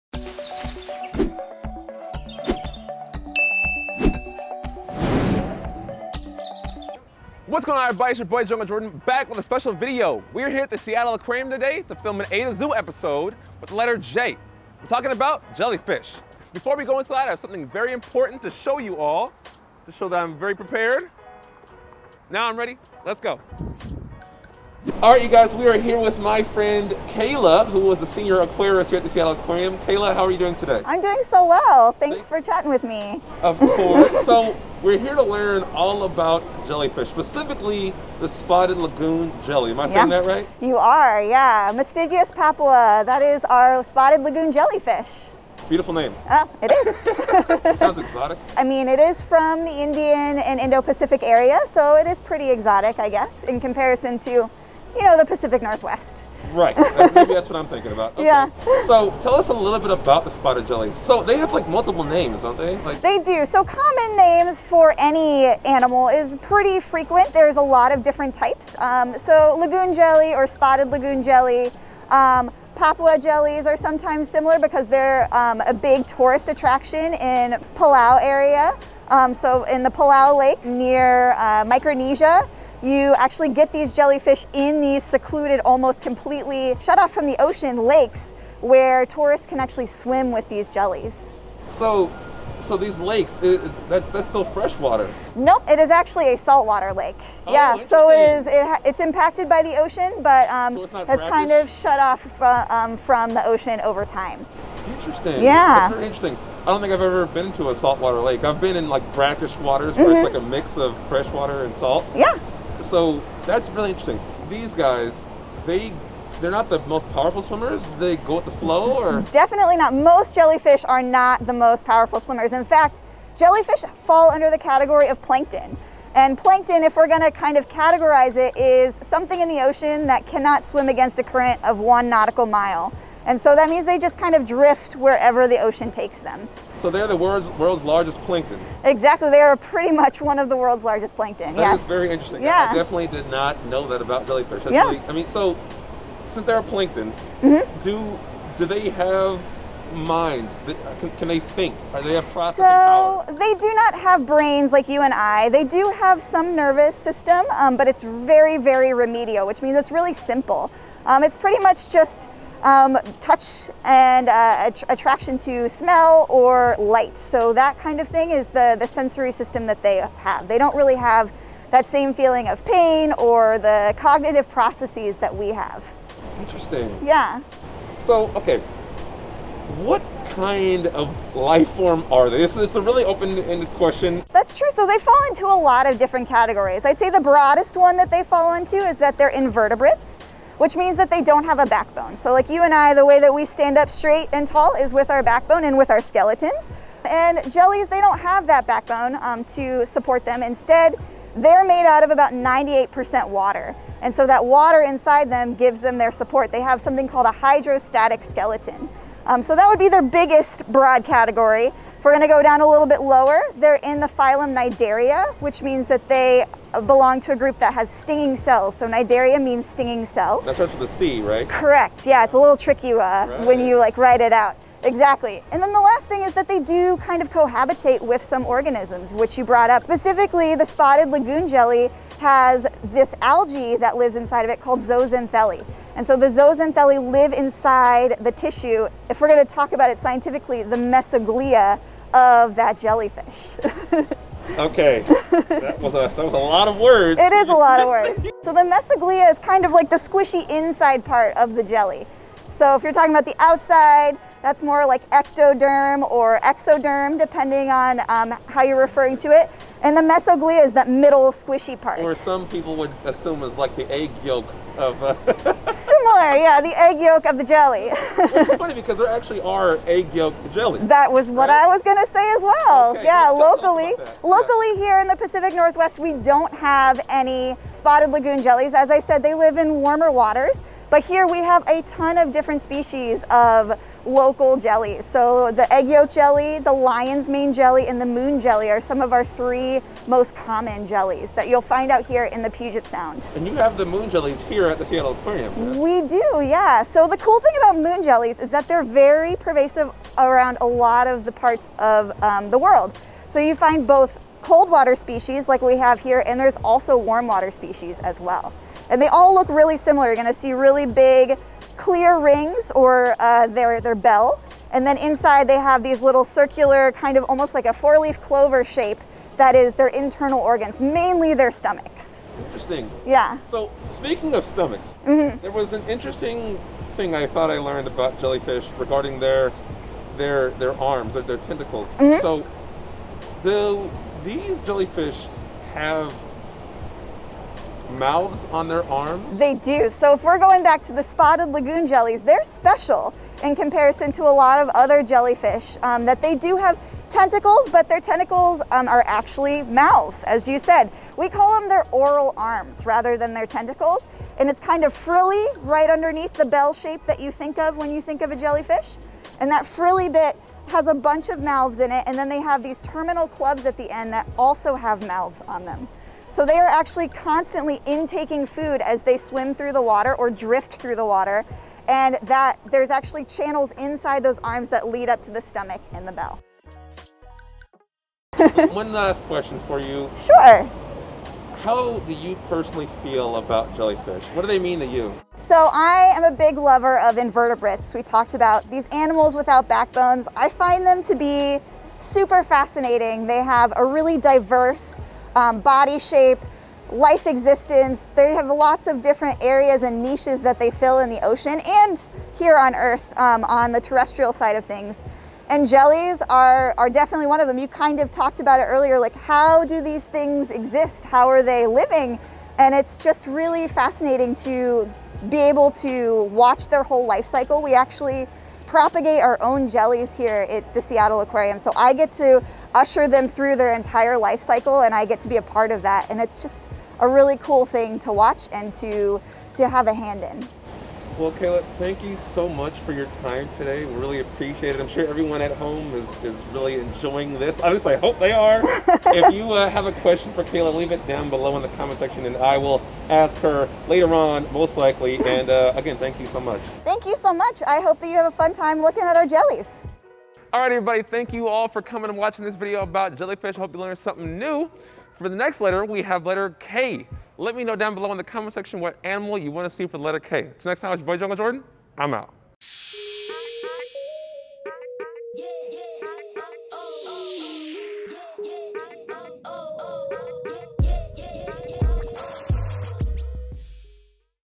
Why are Jellyfish So Confusing?
There were so many big words being tossed around in this A to Zoo video on the letter J. We visited the Seattle Aquarium to learn a little more about the interesting make up of the Spotted lagoon jellyfish.

Filmed by Woodrick Productions